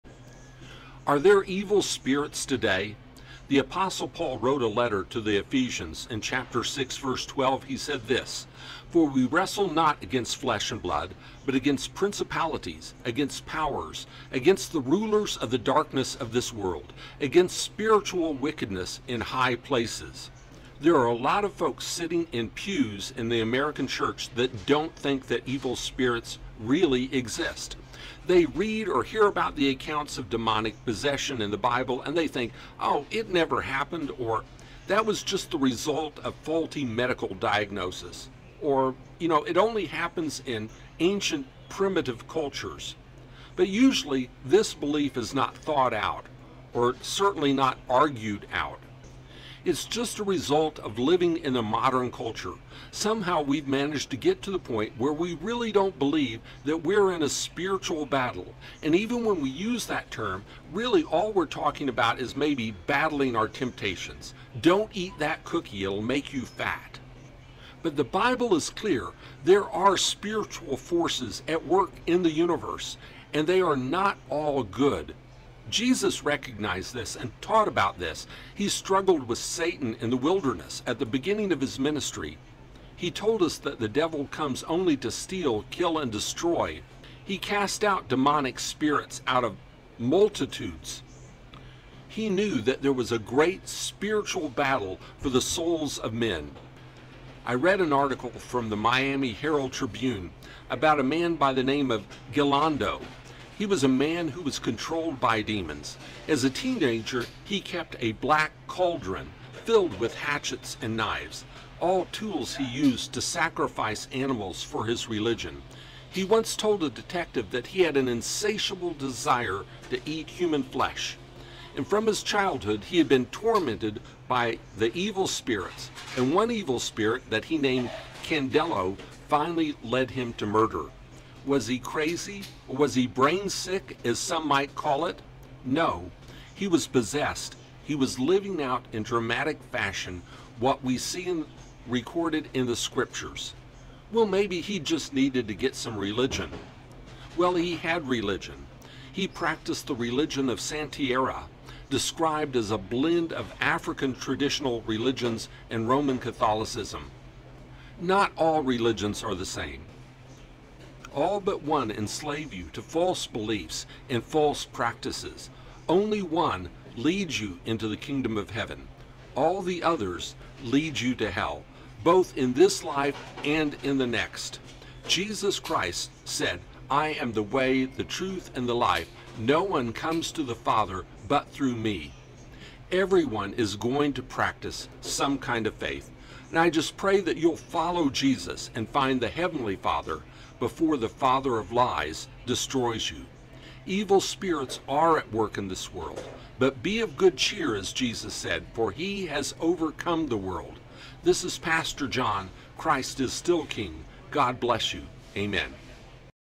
Crazy or Possessed?
Evil spirits surround us. They seek our destruction. What does the Bible teach us about them and how we should protect ourselves?

Try the Bible I use from Amazon.